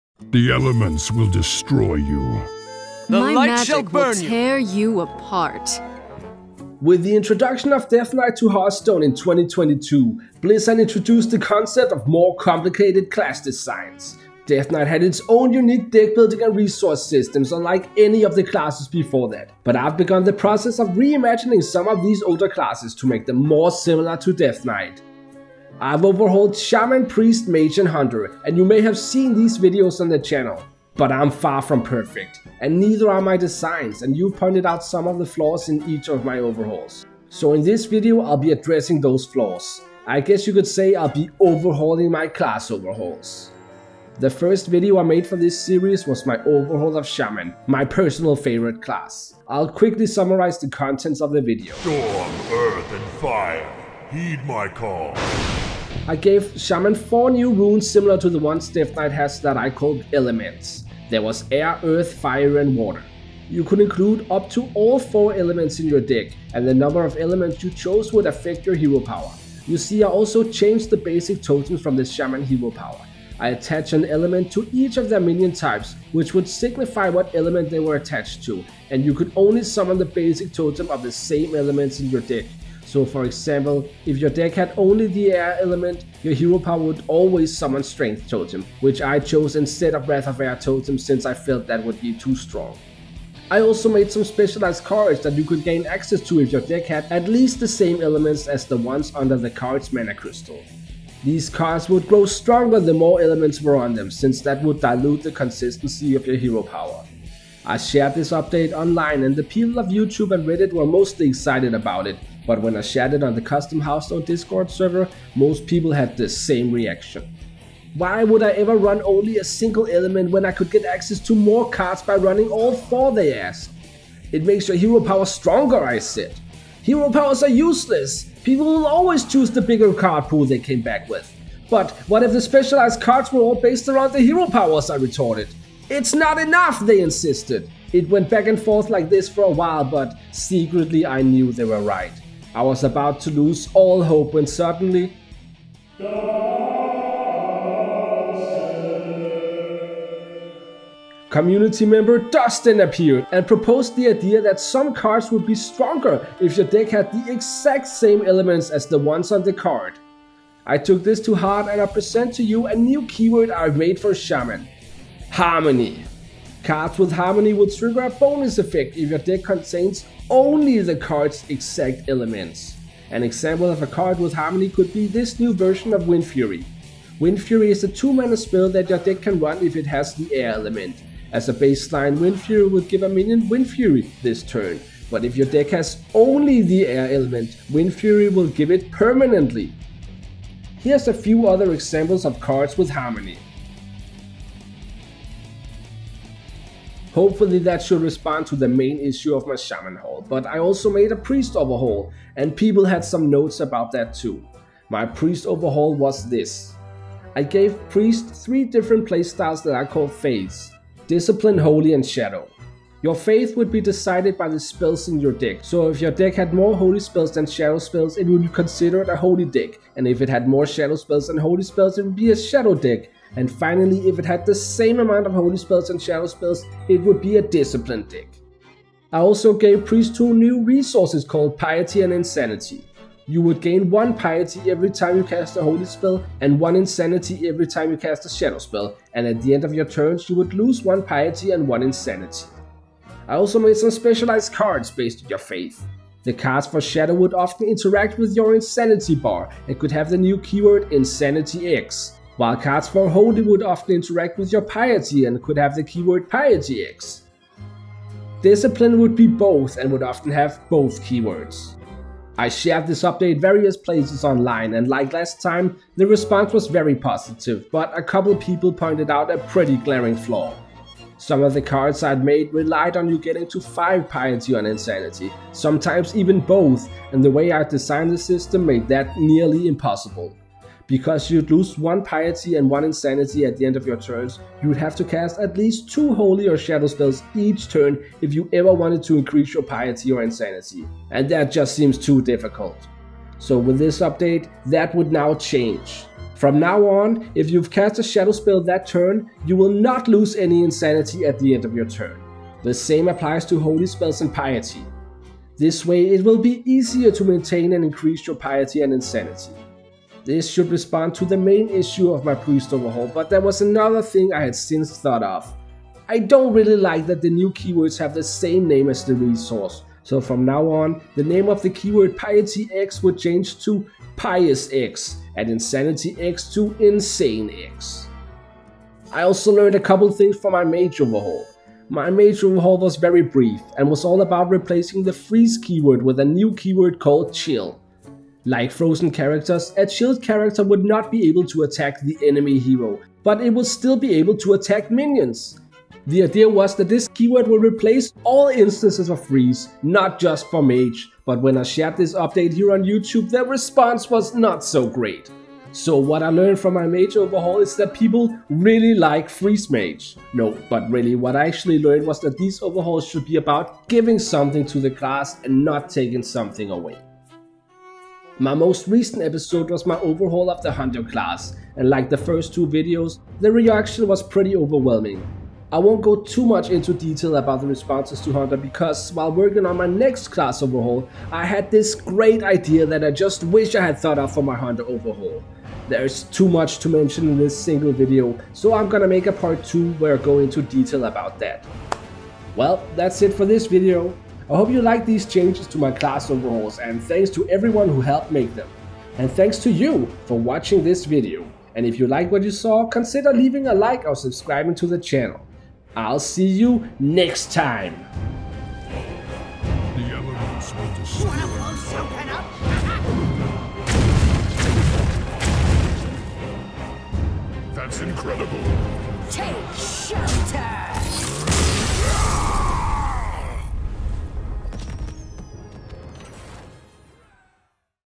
I OVERHAULED my Hearthstone Class Overhauls!
I've overhauled four of Hearthstone's older classes, but my videos weren't perfect... So come along as I address those flaws!

00:00 - Introduction
00:43 - Shaman Overhaul
03:20 - Priest Overhaul
05:29 - Mage Overhaul
06:08 - Ending